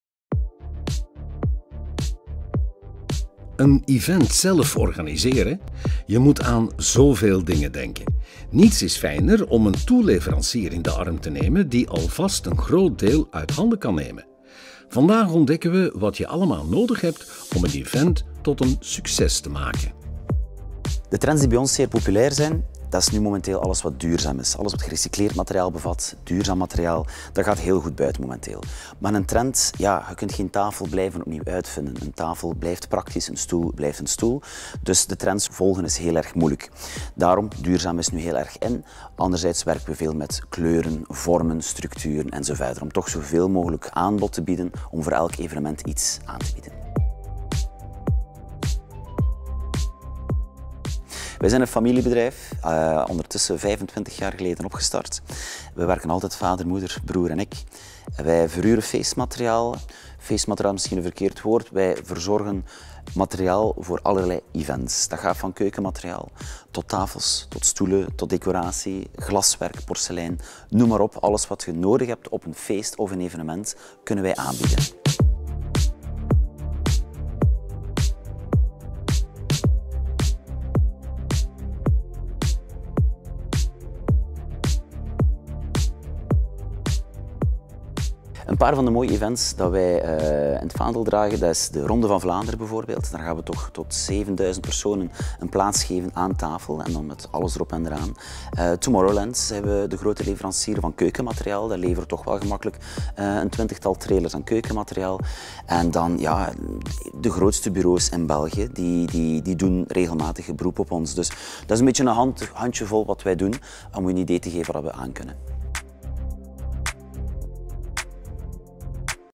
Levi Party Rental
Een event zelf organiseren, je moet aan zoveel dingen denken. Niets is fijner om een toeleverancier in de arm te nemen die dit allemaal uit handen neemt. Vandaag ontdekken we wat je allemaal nodig hebt om een event tot een succes te maken!